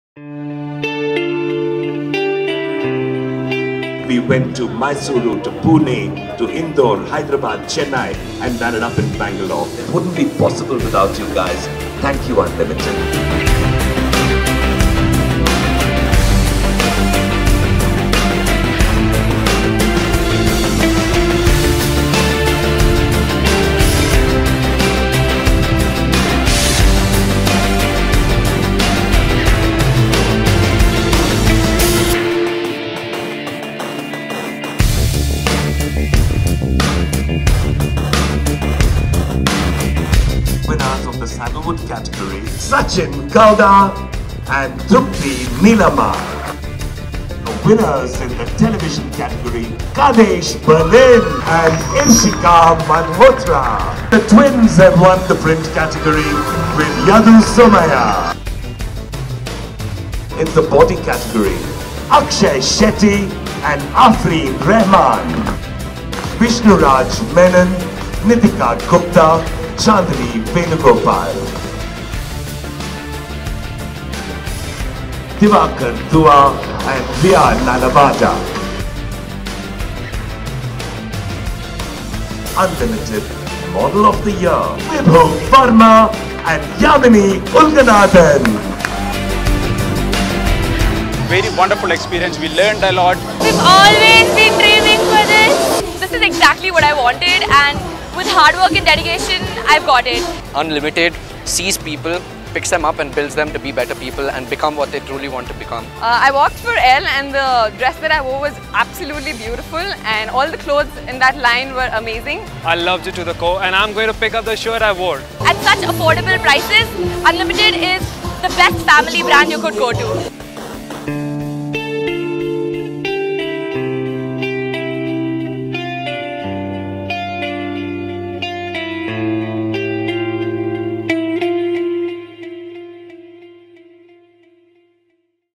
ModelsUnlimited2016 Bangalore Finale Video
All good things come to an end. With this video we part ways with ModelsUnlimited2016. Congratulations all the winners. See you next year with more such great models.